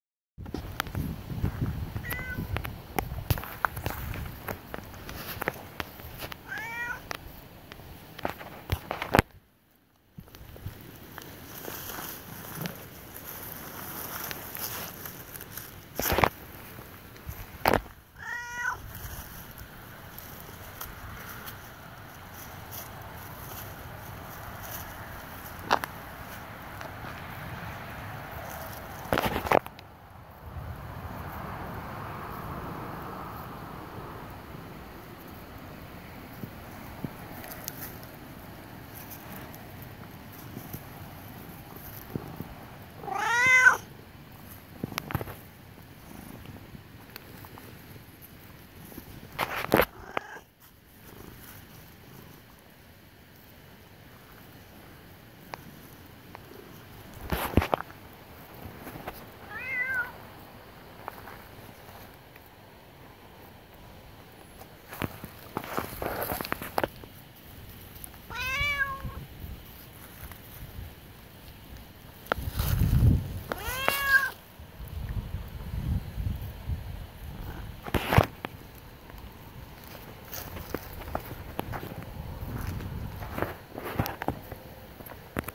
He Was Squatching With Me (Simba Talks A Lot)
Samba was out there with me standing guard while I was Squatching.  He is such an awesome cat.  I also heard him walking along with whatever these things are.  He smelled like wet dog when he got back, also.  It was quite strange. I left them 2 bananas to see if they would take them & apparently something did, because they were gone by the morning time.